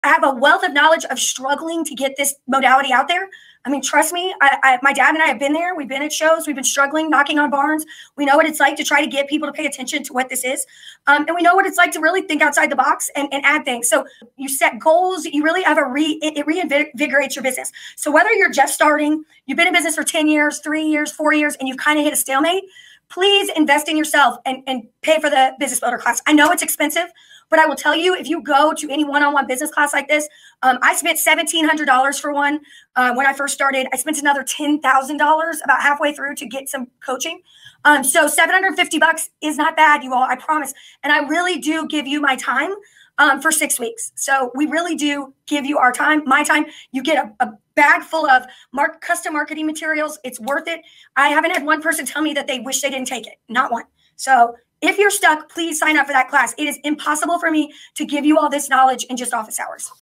Business Builder Classes | MagnaWave
MagnaWave is invested in helping all of our practitioners become successful in their MagnaWave businesses. In order to help them grow to their maximum potential, we offer business building classes to teach practitioners the methods to price their services, approach potential clients and how to improve their reach in their areas of service. The classes are small to allow those who attend the opportunity to ask questions and get the individual instruction they need from MagnaWave president Alane Paulley.

to schedule your class today!